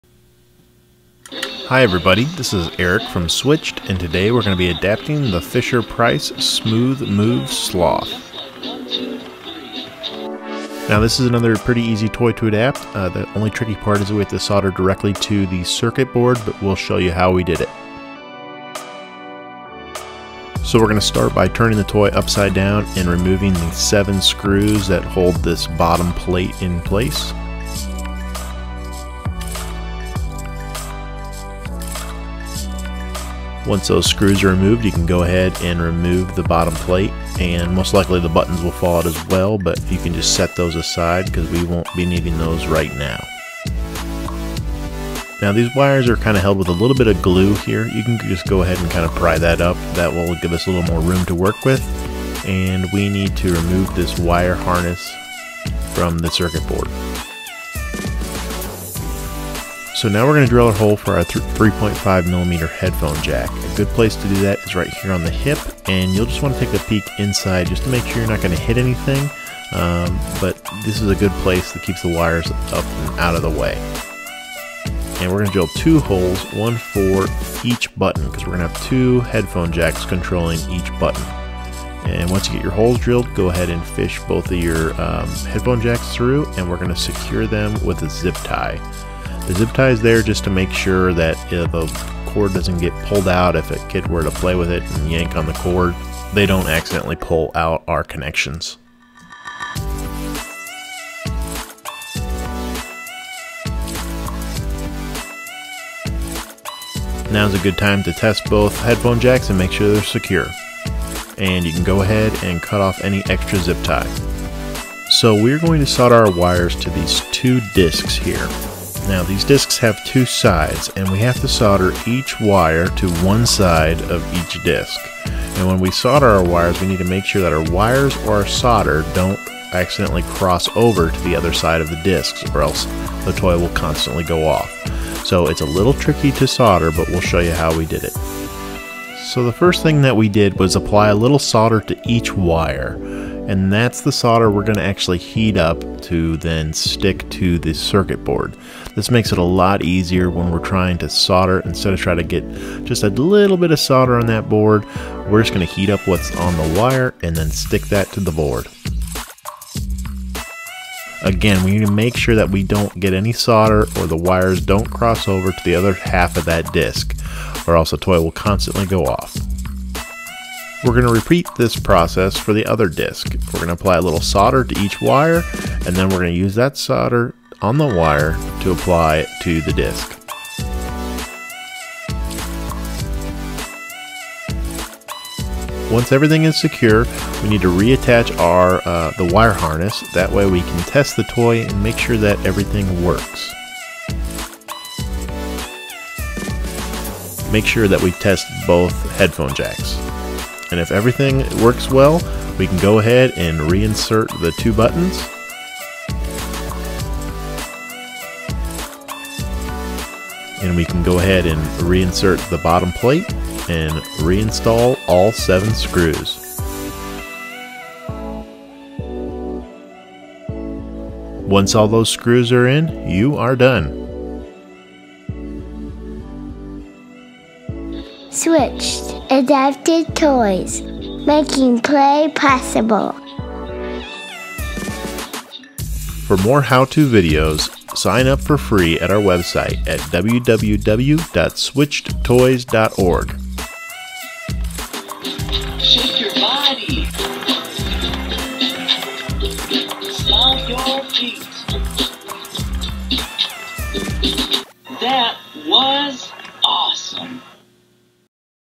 SWITCHED Adapted Toys - Smooth Moves Sloth - Manual
Instruction manual for adapting the Fisher-Price Smooth Moves Sloth to be switch accessible. For more information or additional manuals visit us